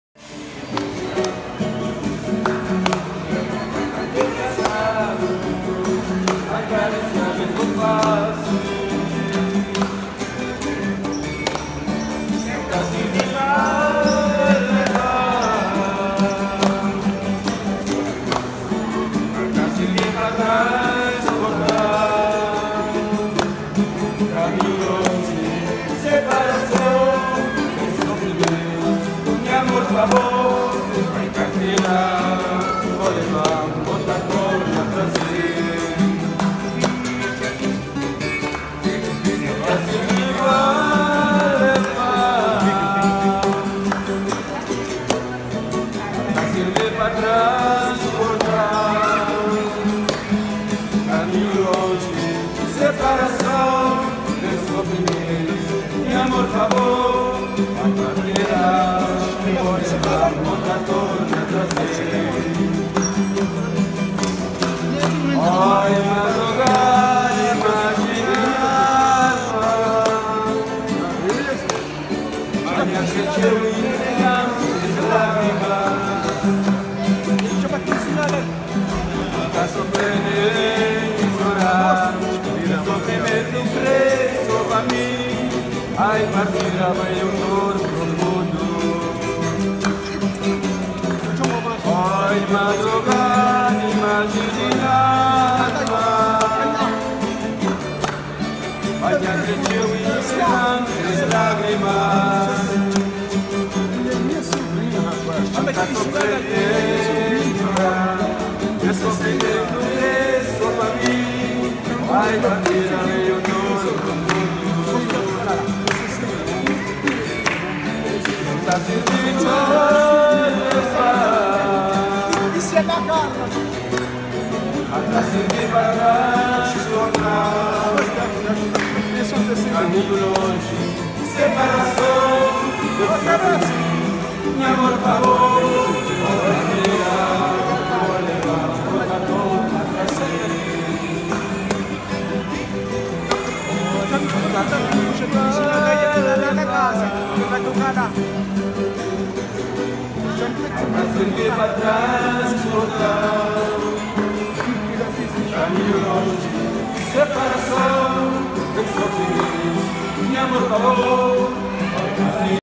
Capeverdean traditional farewell
When a Capeverdean person leaves home for working abroad, family and friends bring them to the airport and the goodbye is not only about hugs, kisses and ties. They bring the guitar and their voices and play traditional goodbye mornas (traditional music).

This traditional farewell occurred in Cesaria Evora Airport, in Mindelo, Cape Verde